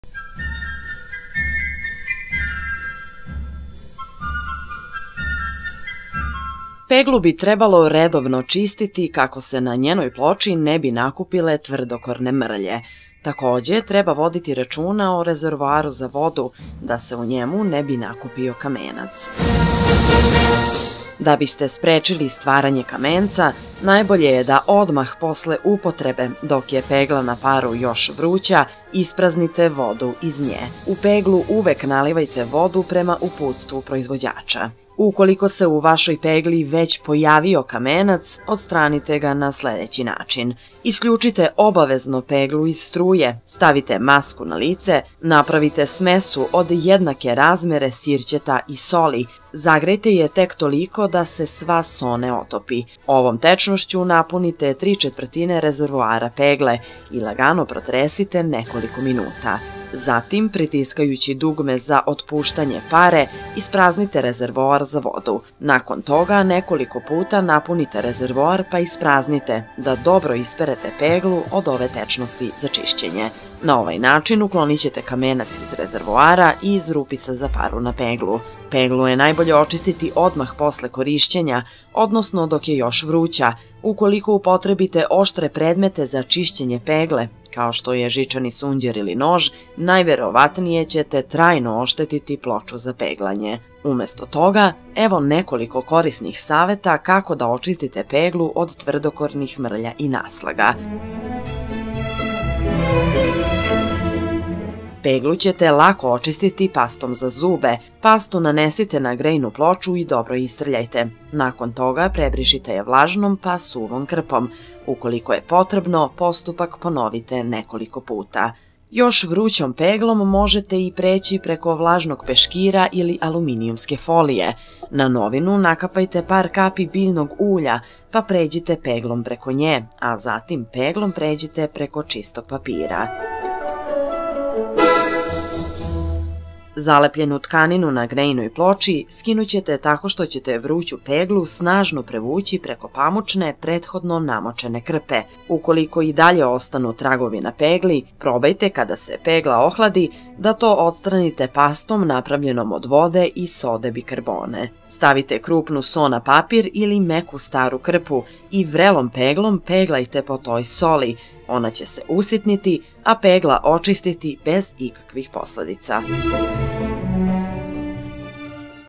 Kako da očistite peglu
U ovom videu saznaćete kako da očistite peglu.